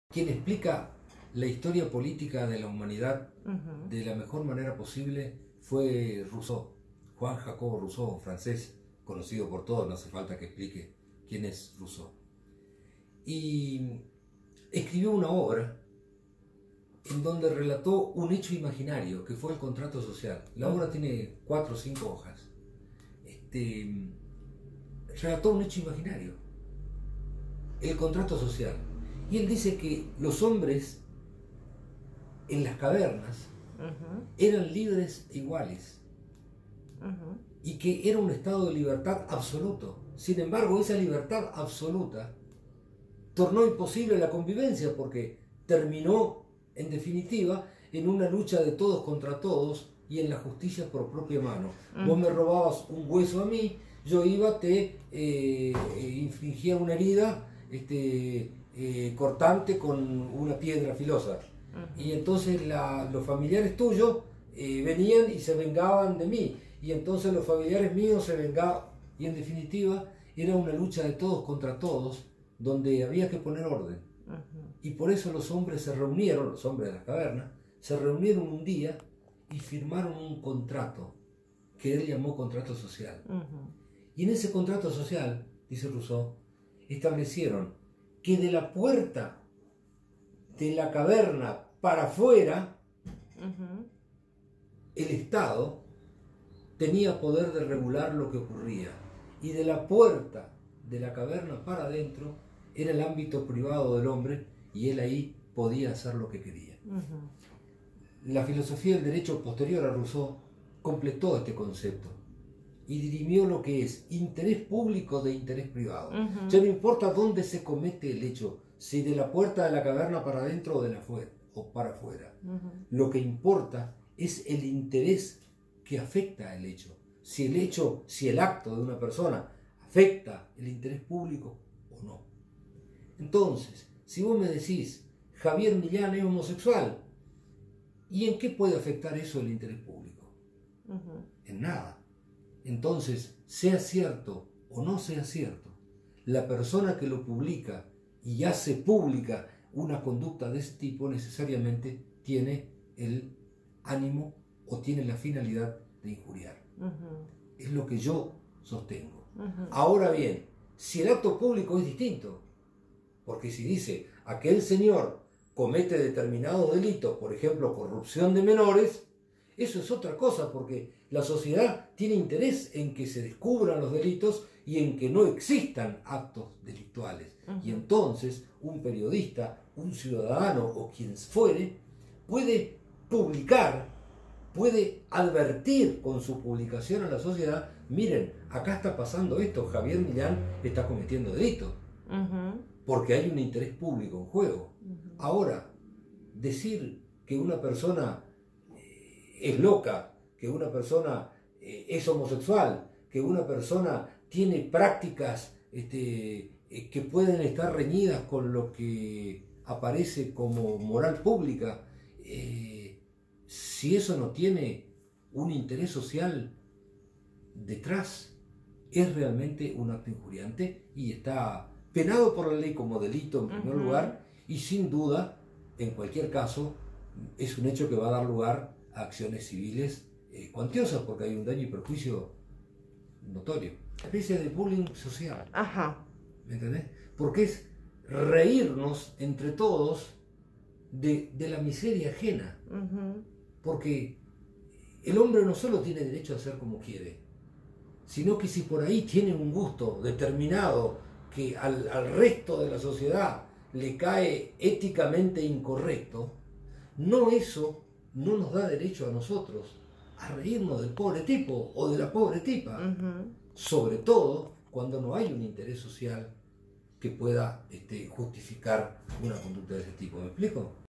Lo que pasó en Oberá “fue una especie de bullying social”
El jueves pasado dejó de existir Carlos Glitz, el hombre de 50 años, cuyo cuerpo fue hallado debajo del puente que une Garupá y Candelaria. Glitz, era intensamente buscado desde el lunes 12, momentos en que se ausentó de su domicilio y sus familiares habían radicado una denuncia por desaparición. Según trascendió, el hombre se habría quitado la vida producto de escraches en la red social Facebook.